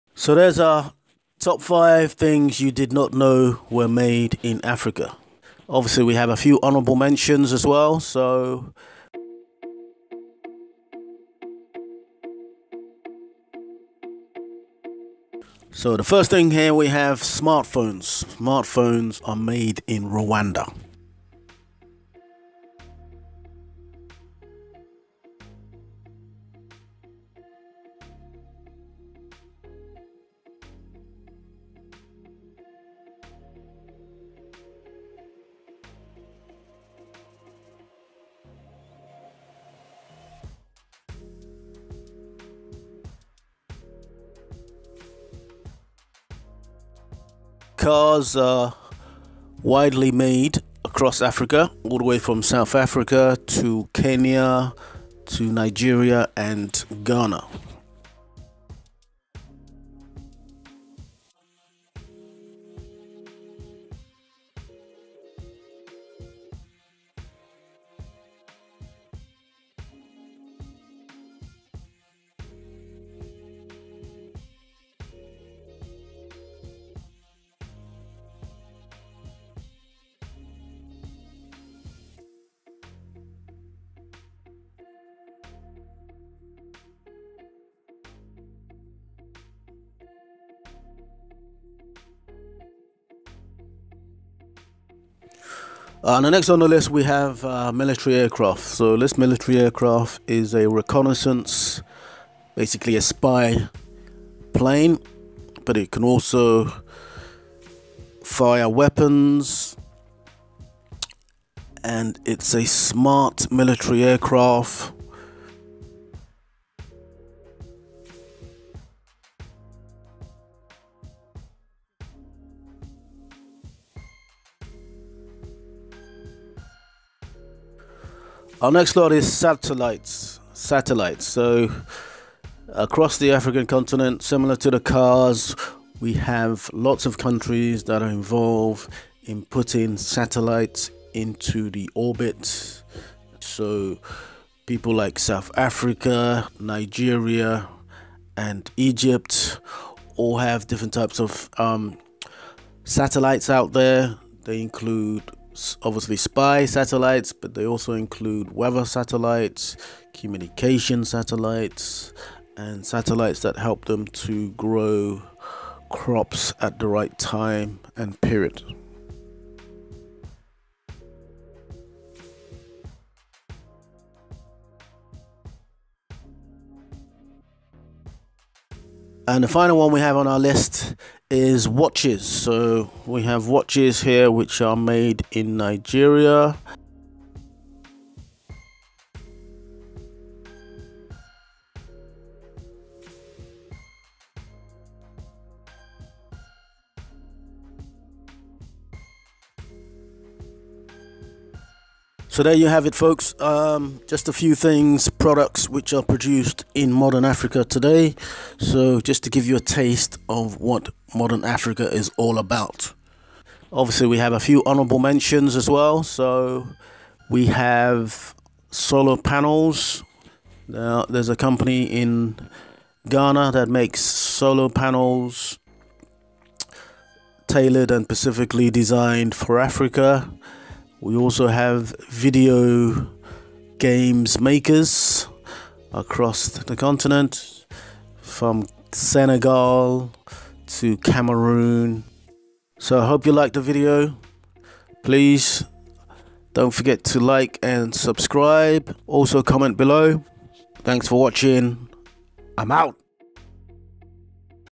5 THINGS THAT ARE MADE IN AFRICA | FROM CARS, PLANES TO MOBILE PHONES MADE IN MODERN AFRICA
5 THINGS THAT YOU DID NOT KNOW ABOUT MODERN AFRICA THAT INCLUDE THINGS WHICH ARE MANUFACTURED AND MADE IN AFRICA. COUNTRIES LIKE UGANDA, KENYA, EGYPT, NIGERIA, GHANA, SOUTH AFRICA, TUNISIA, RWANDA AND MOROCCO.
NIGERIA PRODUCES CARS, BUSES, TRUCKS, SATELLITES, AND WATCHES.
GHANA CREATES SOLAR PANELS AND CARS
RWANDA MAKES MODERN SMART PHONES

WHILE SOUTH AFRICA, UGANDA, MOROCCO AND KENYA ALL MAKE AUTOMOBILES.
SOUTH AFRICA ALSO MAKES MILITARY AIRCRAFTS AND WINE.
SENEGAL AND CAMEROON PRODUCE VIDEO GAMES FOR PC AND MOBILE PHONES

POK DA BER

Collaborations welcomed:
We TRAVEL everywhere